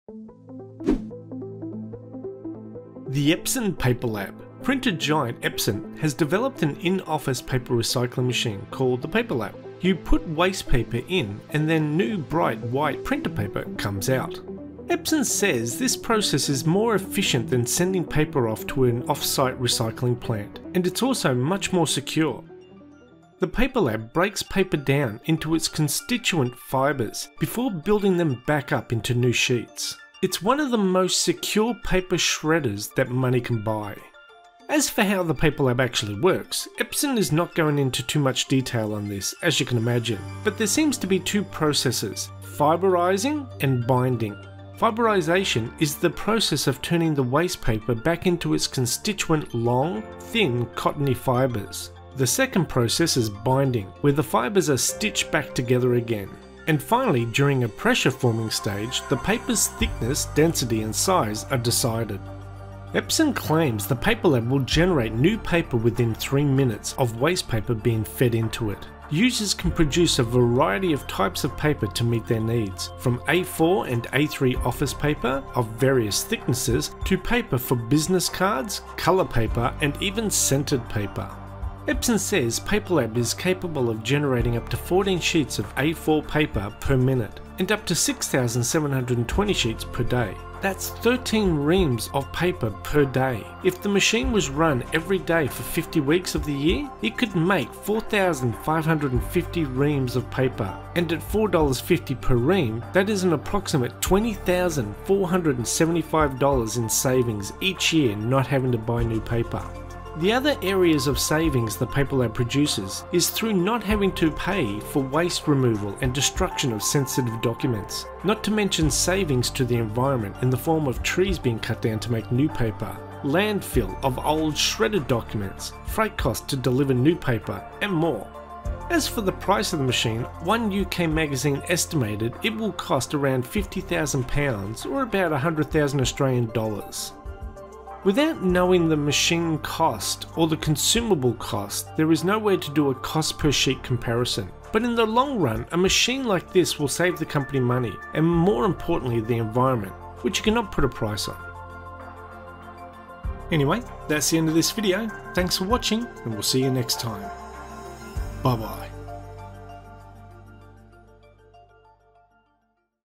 Epson PaperLab - Dry Process Paper Recycle In-Office Machine
Epson PaperLab A-8000 - is an in-Office Paper Recycling Machine, considered the best Paper Shredder money can buy.

It is also the World First Dry Process Paper Recycle Machine.

Printer giant Epson has developed an in-office paper recycling machine, called the PaperLab A-8000, you put waste paper in and then new, bright white printer paper comes out.

Epson says Paperlab is capable of generating up to 14 sheets of A4 paper per minute and up to 6,720 sheets per day. That’s 13 reams of paper per day. If the machine was run every day for 50 weeks of the year, it could make 4550 reams of paper and at $4.50 per ream that is an approx $20,475 dollars in savings each year not having to buy new paper.

CREDIT LINKS